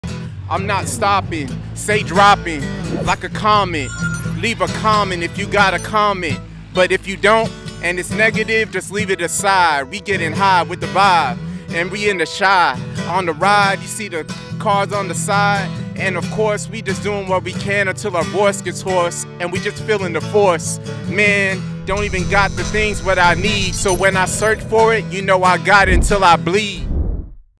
Acoustic Cyphers ep2 MY
Acoustic Cyphers Resident Emcee MY spitting for the people in the streets!